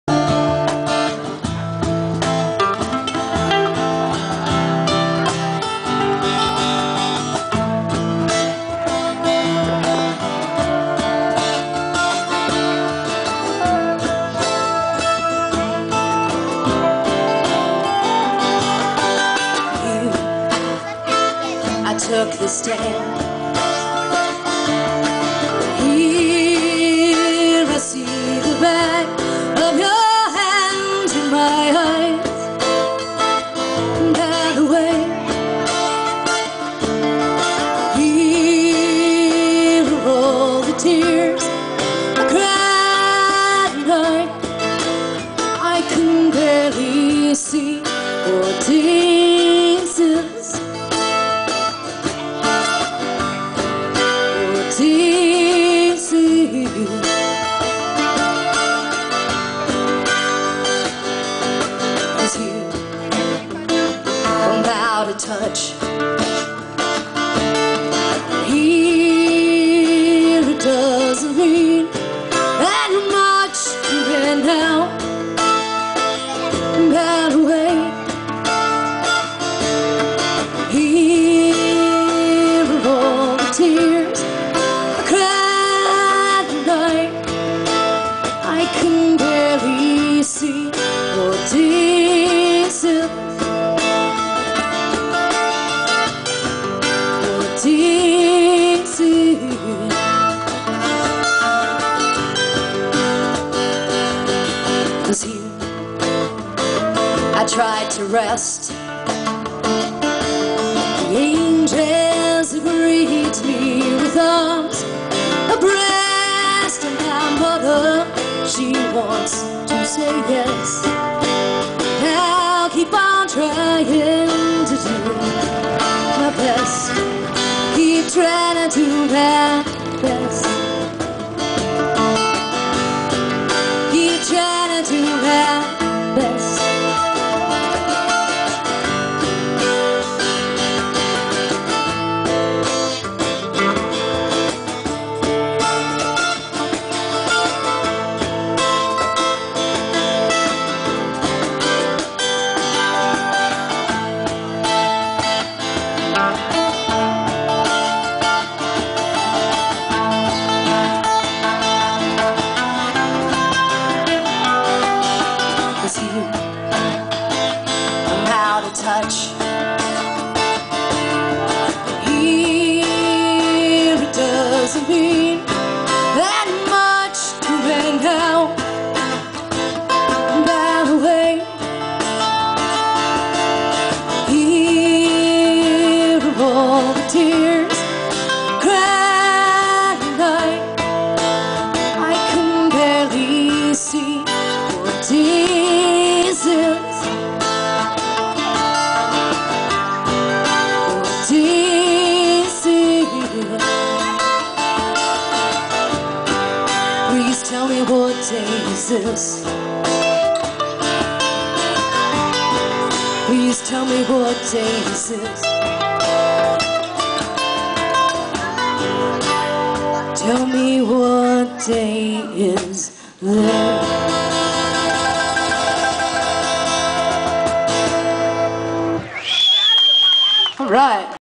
Robin Kirby
Here live @ Clifton's Seaside
Robin Kirby, Peter Gruezmacher, Laura Gillis, Mark Zues, and Otis